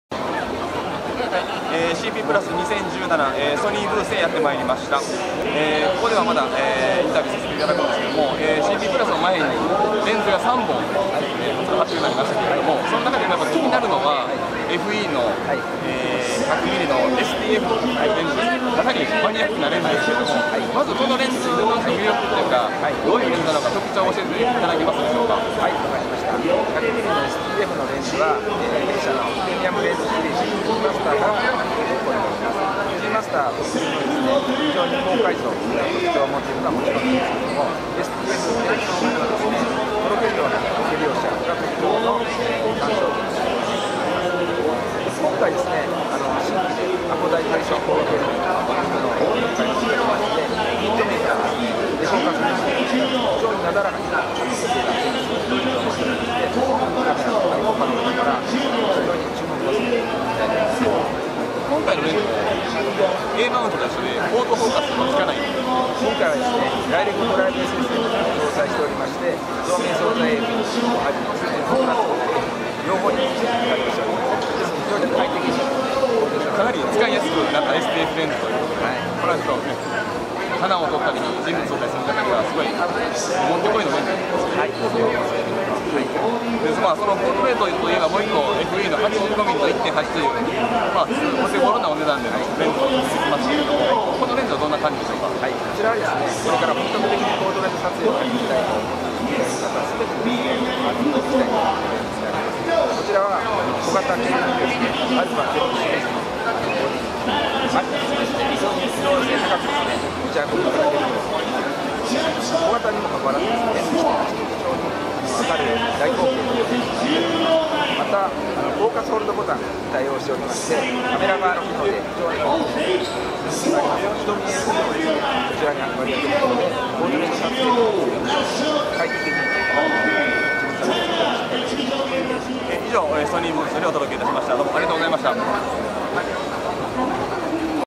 【CP+2017】ソニー動画レポート（カメラのナニワ／レモン社）
CP+2017動画レポート・ソニーブース編。カメラのナニワ・レモン社のナニワグループの動画レポート。各社ブースの担当の方に直撃でいろいろお伺いしました。